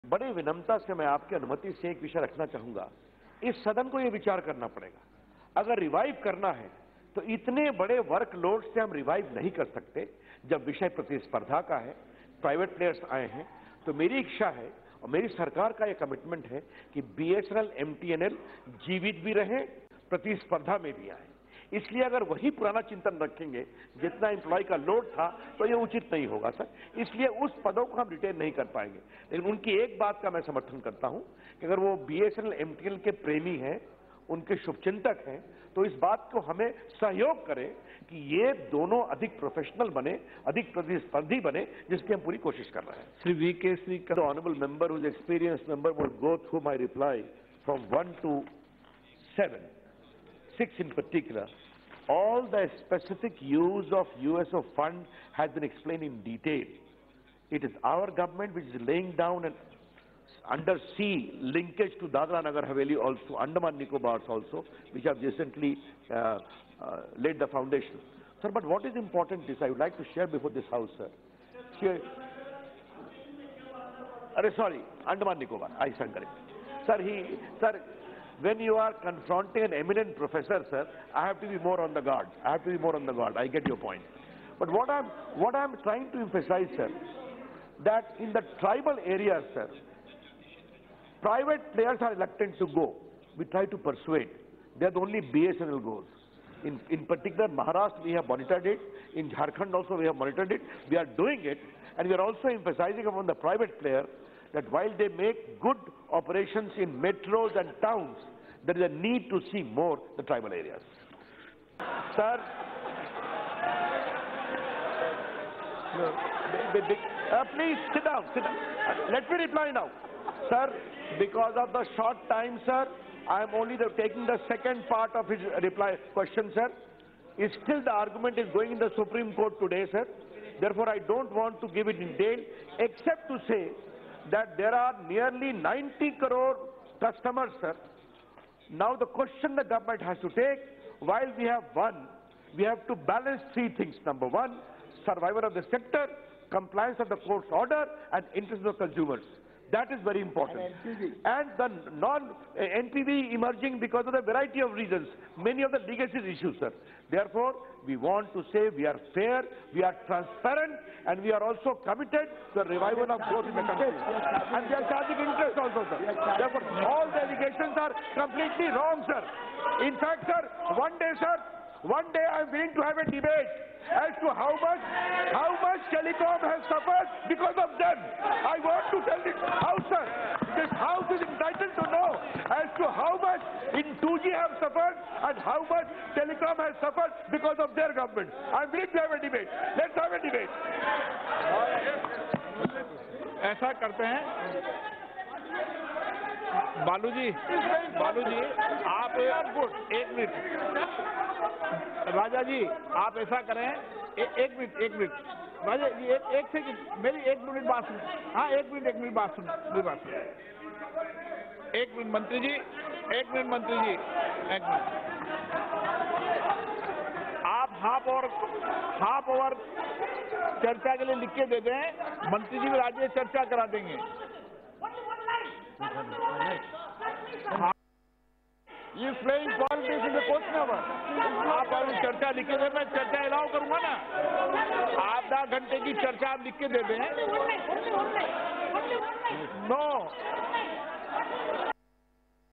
Ravi Shankar Prasad Says govt has Decided to Revive BSNL and MTNL | NTV
Ravi Shankar Prasad Says govt has Decided to Revive BSNL and MTNL